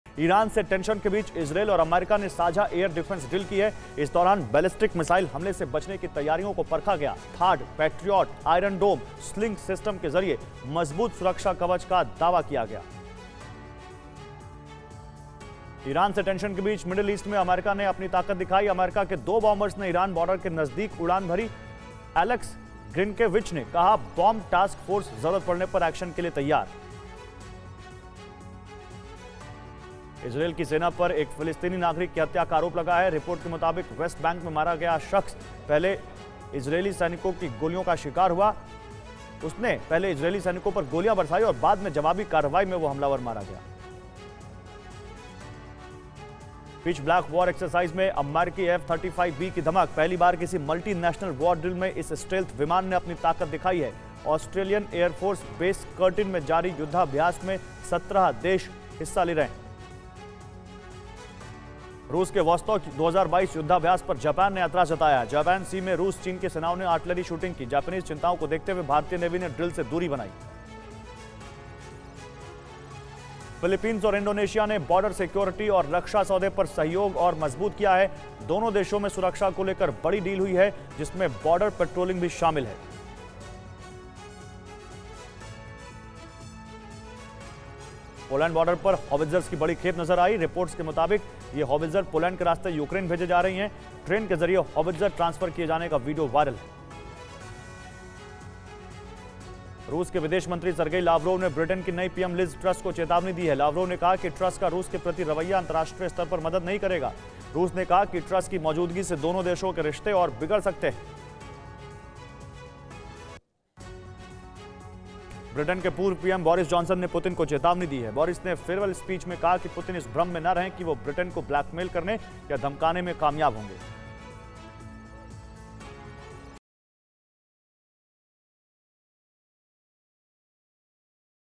Iran से Tension के बीच Israel और America ने साझा की Air Defense Drill | Biden | Putin | Russia
ईरान से टेंशन के बीच इजरायल और अमेरिका ने साझा एयर डिफेंस ड्रिल की। इस दौरान बैलिस्टिक मिसाइल हमले से बचने की तैयारियों को परखा गया। थाड, पैट्रियोट, आयरन डोम, स्लिंग सिस्टम के जरिए मजबूत सुरक्षा कवच का दावा किया गया।
Israel and the US conducted a joint air defense drill amid tensions with Iran. During this, preparations to avoid ballistic missile attack were tested. Strong defenses claimed through THAAD, Patriot, Iron Dome, Sling system.